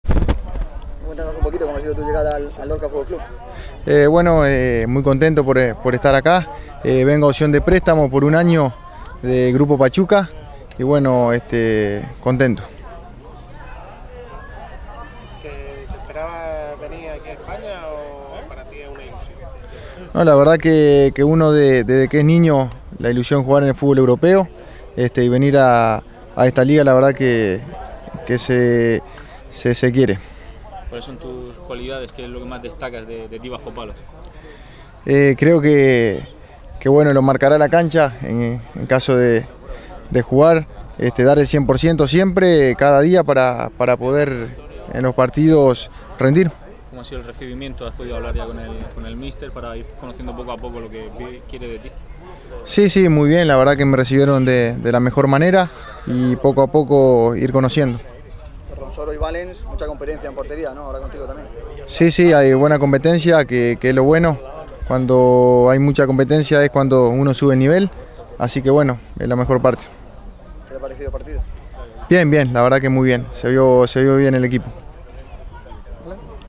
FRANCO TORGNASCIOLI
El Lorca Fútbol Club ha cerrado un acuerdo con Franco Torgnascioli para que este juegue en el Lorca FC durante la próxima temporada. Torgnascioli es un portero uruguayo de 26 años que llega procedente del equipo mejicano de Mineros de Zacatecas, donde la pasada temporada jugó 40 partidos.
Anteriormente, Franco Torgnascioli defendió la camiseta de Danubio, de Primera uruguaya y en su país es considerado uno de los mejores porteros. Franco Torgnascioli ya está en la concentración del equipo tras superar las pruebas médicas y atenderá esta tarde a los medios de comunicación tras la disputa del encuentro que el Lorca FC va a disputar en Pinatar Arena ante el Mar Menor a partir de las 19:00 horas.
Torgnascioli llega de la mano del representante de jugadores y ex futbolista del Lorca CF, James Cantero.